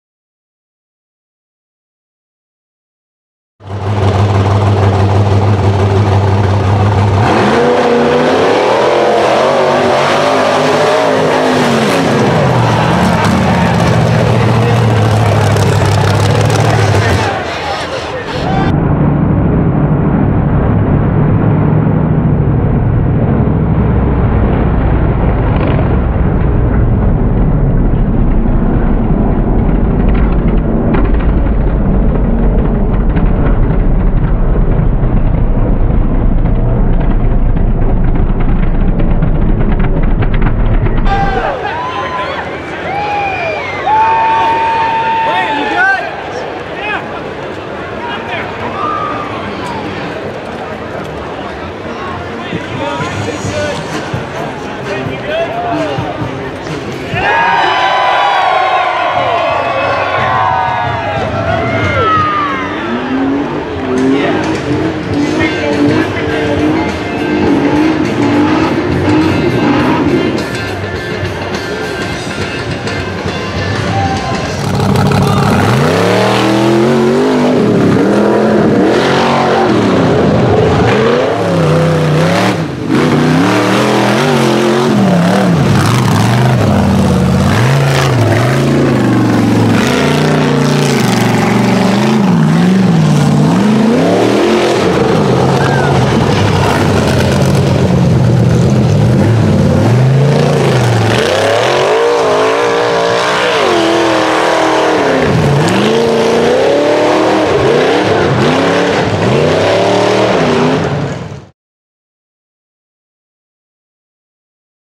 THE GENERAL GOES FOR IT and CRASHES HARD
Clint Evans tried his best to jump the step up on the 2nd hill at the SRRS race. Unfortunately for Clint it didn't end up well and he pretty much lawn darted right into the ledge of the 2nd hill. Thankfully the safety equipment did it's job and Clint was fine. I am pretty sure he was sore the next day though.